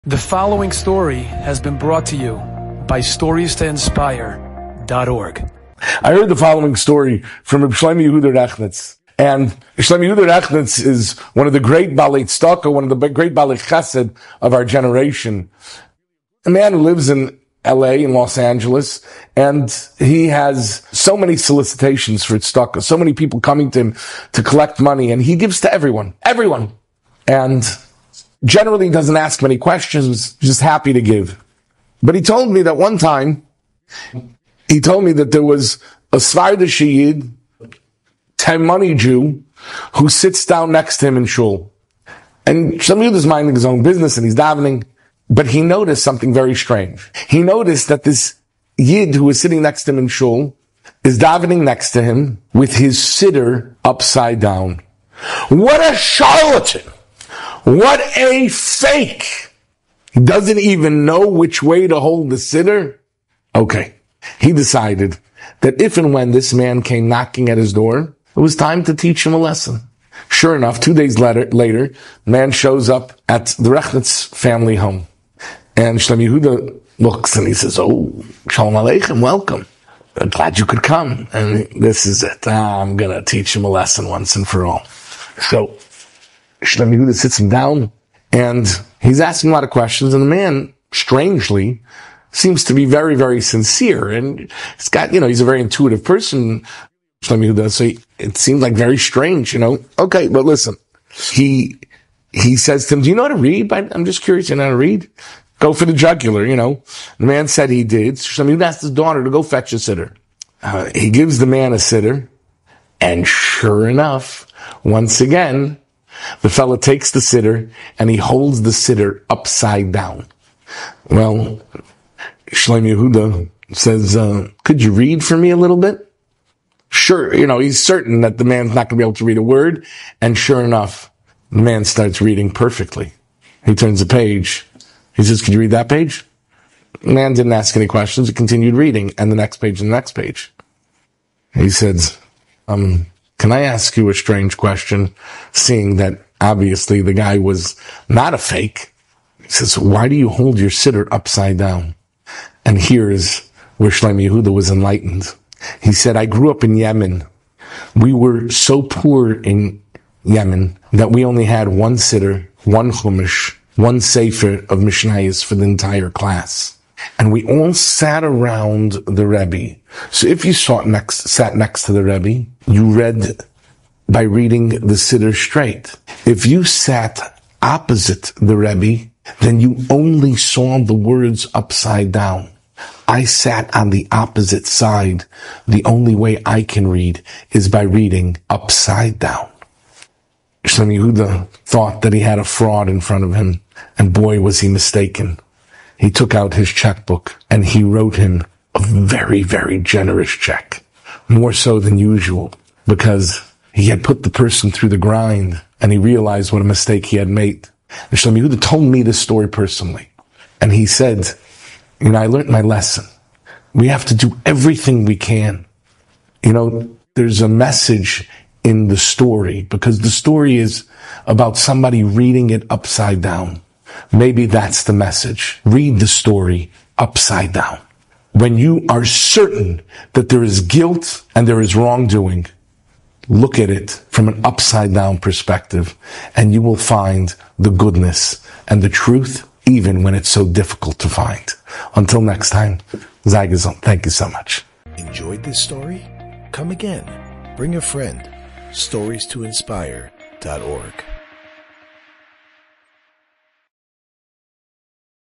An Upside Down Perspective - A Story about Shlomo Yehuda Rechnitz - Rabbi Yechiel Spero
R' Yechiel Spero

To listen to our hotline, please call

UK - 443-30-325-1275